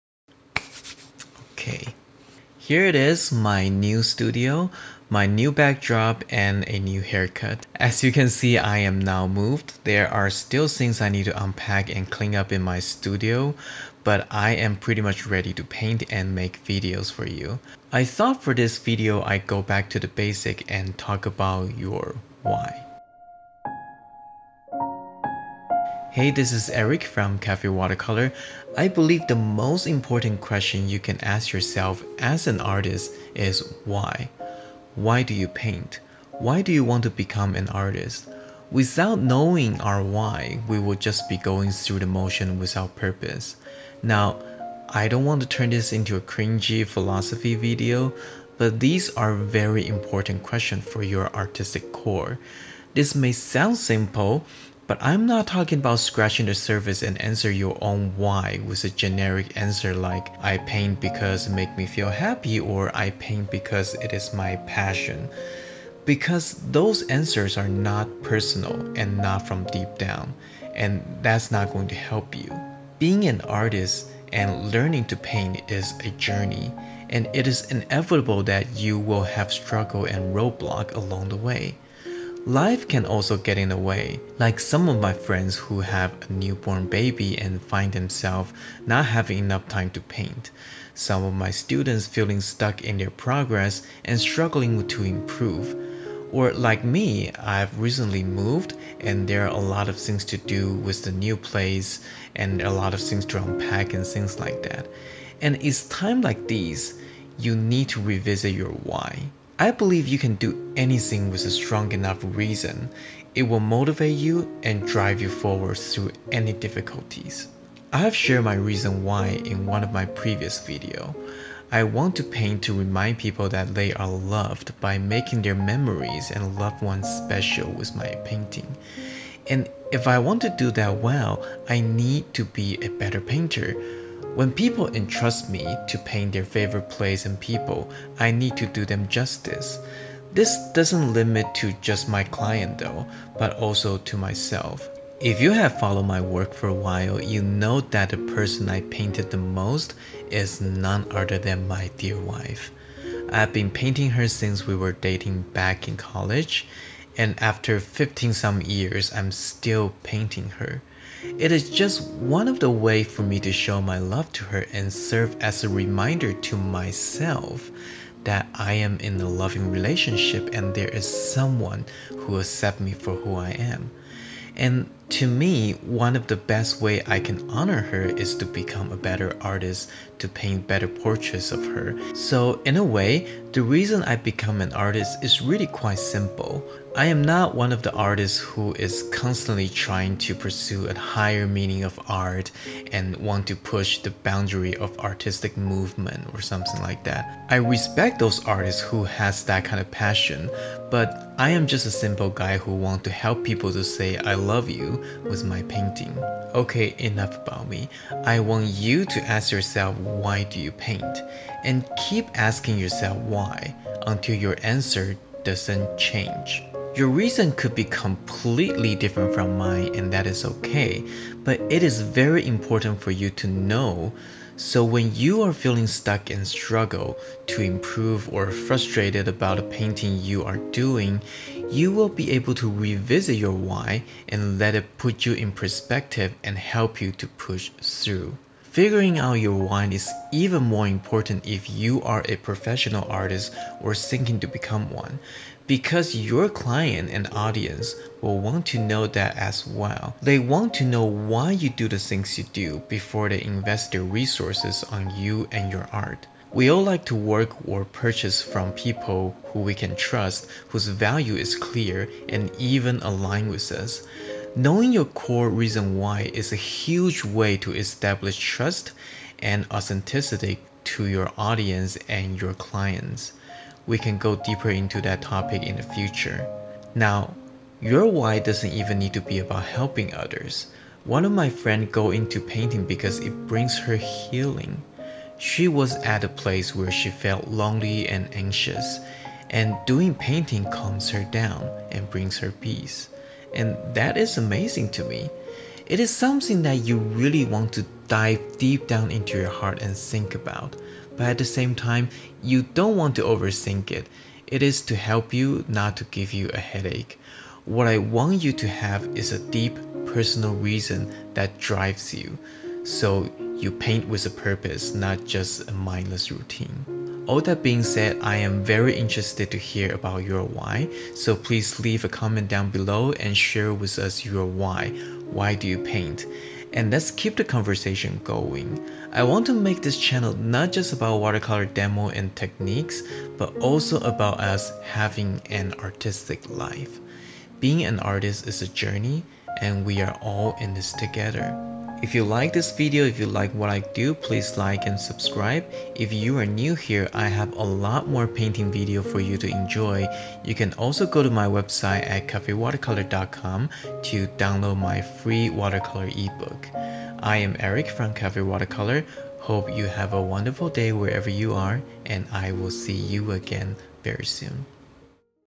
DON'T commit to painting before you answer this question - your WHY
As an artist, it is very important to know your core reason WHY. Having a personal and strong enough reason, you will be able to push through during difficult and struggling time.
Let me know what is your WHY, and let's keep the conversation going! =)